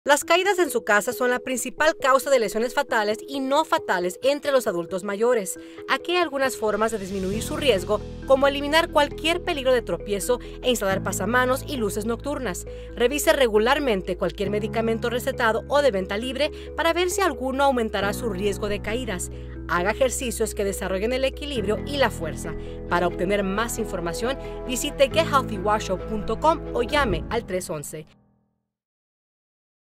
Prevención de caídas en personas mayores
Las caídas sin intención representan la causa más alta de traumas reportados en el Condado de Washoe y en Nevada, y la causa principal de muertes relacionadas con heridas para los mayores estadounidenses de 65 años (CDC). La mayoría de las caídas involuntarias ocurren dentro de la residencia del hogar. Aquí hay tres pasos simples que usted puede hacer para usted o sus seres queridos para ayudar a prevenir las caídas

Proteja su casa contra las caídas: Haz modificaciones sencillas en tu casa para evitar las caídas

o Elimina todos los peligros de tropiezo, como las alfombras. Mantenga todos los pasillos limpios y ordenados.
o Instalar pasamanos y barras de apoyo en los baños y las escaleras.
o Utilizar alfombras que no resbalen.
o Instala luces nocturnas en los dormitorios, baños y pasillos. Ilumine sus espacios vitales. Utilice esta lista de chequeo para la prevención de caídas en el hogar para personas mayores

Para obtener modificaciones de seguridad en el hogar que cumplan los requisitos de ingresos en el norte de Nevada, póngase en contacto con

o Rebuilding Together Northern Nevada
Programa de hogares seguros y saludables

o Care Chest Northern Nevada
Vida autónoma y prestamo CARE

o Catholic Charities of Northern Nevada:
Modificación de la vivienda para personas mayores

Revise regularmente los medicamentos de venta libre, los suplementos dietéticos y los medicamentos recetados con su farmacia o proveedor de atención médico. Muchos medicamentos y suplementos dietéticos son una causa común de caídas, o de heridas por caídas.

Para obtener evaluaciones gratis de la medicina geriátrica para los residentes del condado de Washoe, póngase en contacto con

Dirección: 6130 Plumas Street, Reno, 89519

Dirección: 1664 N. Virginia, Mail Stop 146, Reno, 89557

Practica habitualmente ejercicios que aumenten el equilibrio, la fuerza y la movilidad, como Stepping On o Fit & Strong.

Clases de ejercicio gratuitas o de bajo costo están disponibles para las personas mayores en el Condado de Washoe, NV

o Sanford Center for Aging
Programa de bienestar para la comunidad